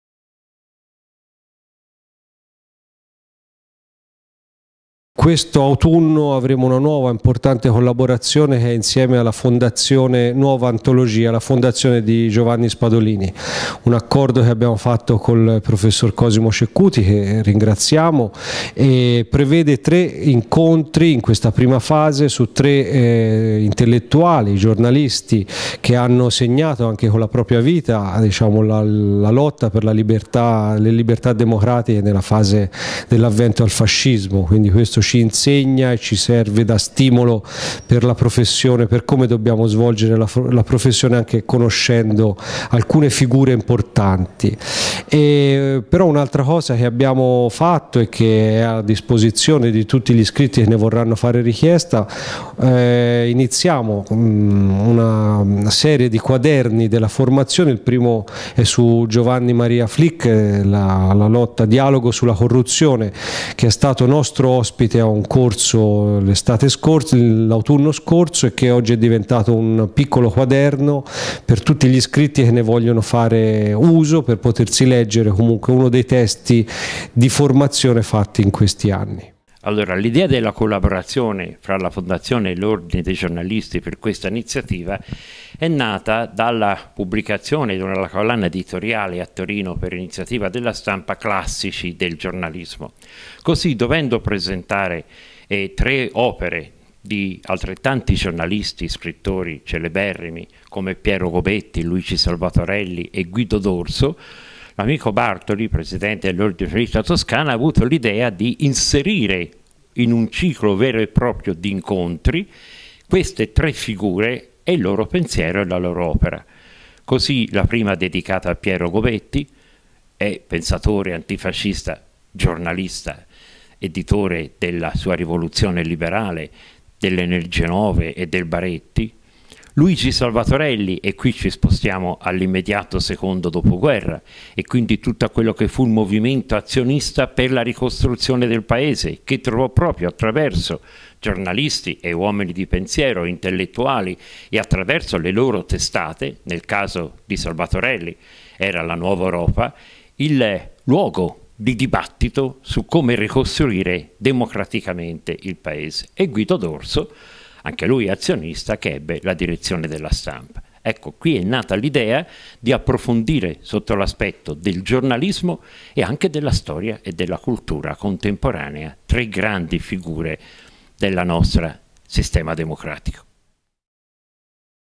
Formazione giornalisti: accordo OdgToscana e Fondazione Spadolini
Per la formazione obbligatoria dei giornalisti l'Ordine della Toscana ha stretto un accordo con un'altra prestigiosa istituzione culturale italiana la Fondazione Spadolini Nuova Antologia diretta dal professor Cosimo Ceccuti.